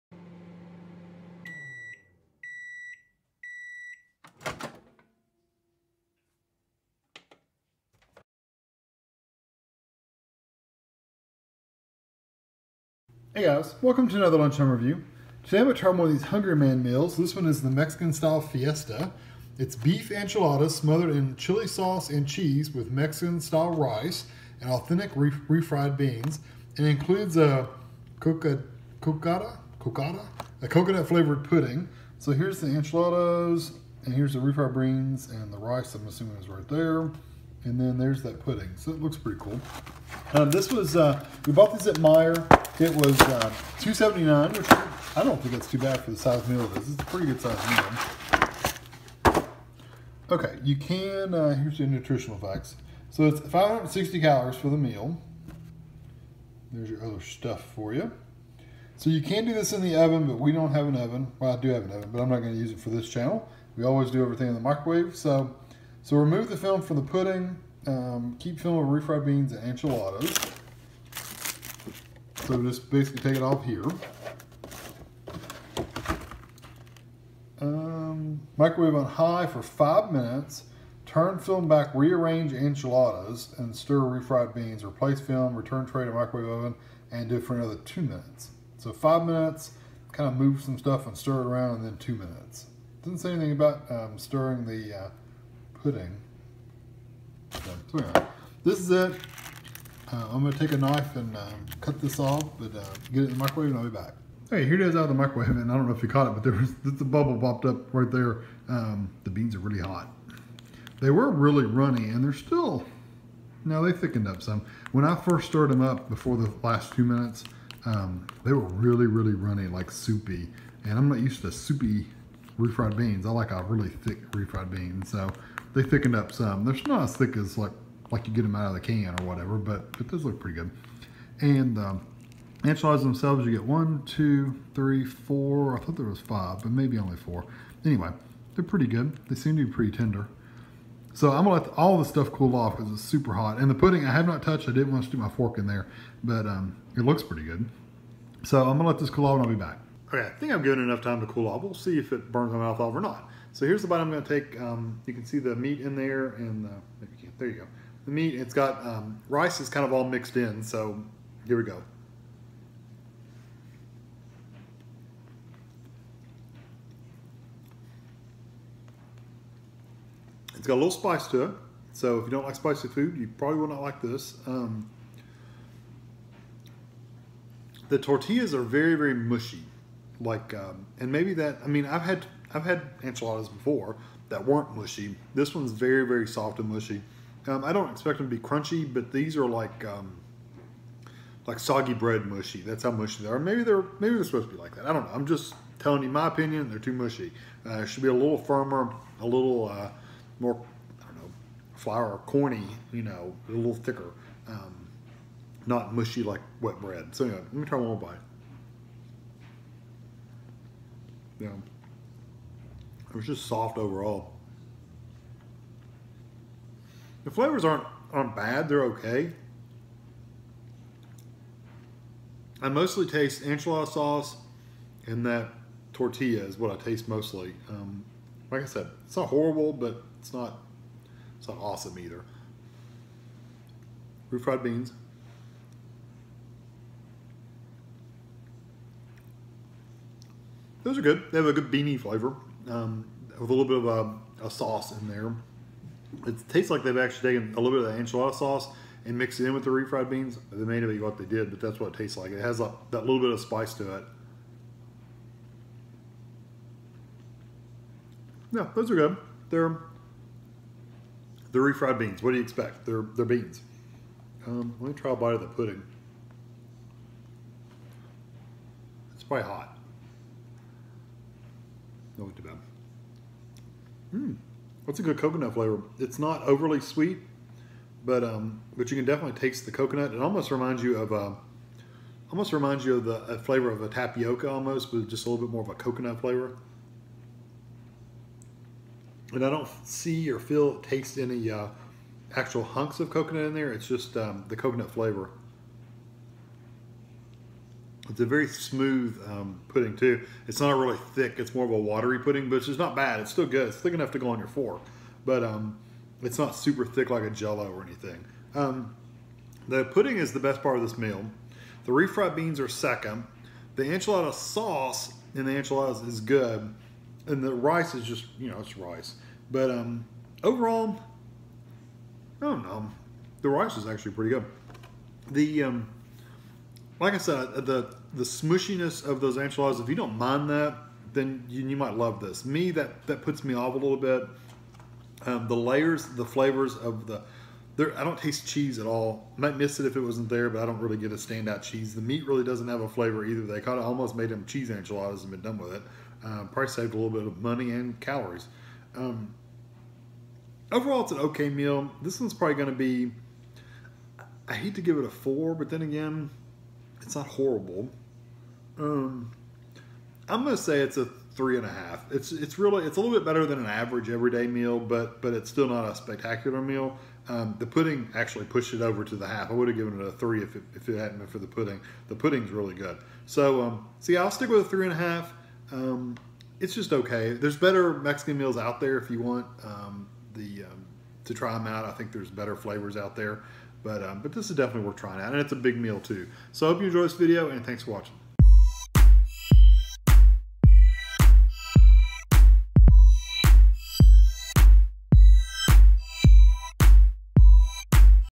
Hungry-Man: Mexican Style Fiesta Review
This is a taste test/review of the Hungry-Man Mexican Style Fiesta frozen meal. It is beef enchiladas smothered in chili sauce and cheese with Mexican style rice and authentic refried beans – includes cocada (coconut flavored) pudding. 1 package (482g) = 560 calories